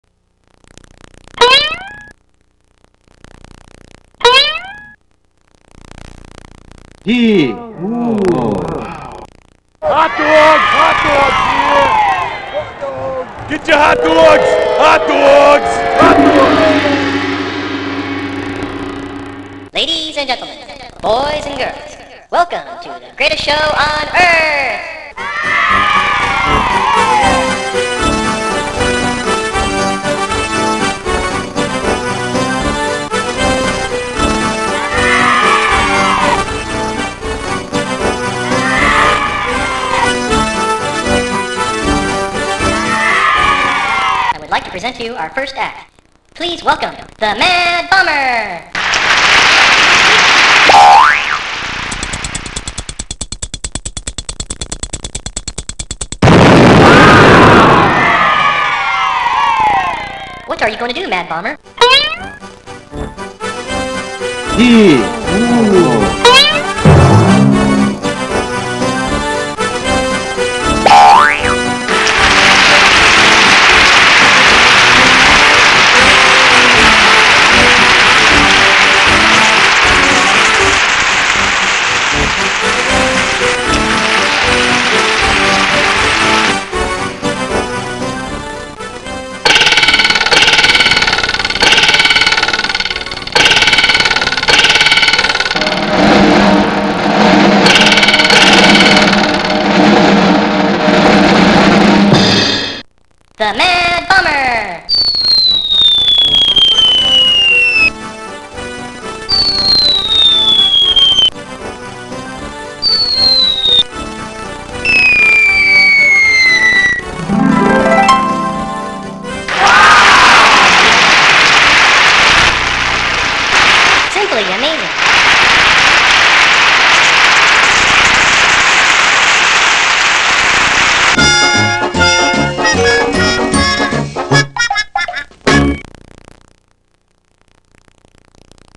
The Mad Bomber - The Great Dive!
See the Mad Bomber jump off a diving board from way up high into a bucket of water at a circus. This rough animation was created in 2001 by csstudio.biz.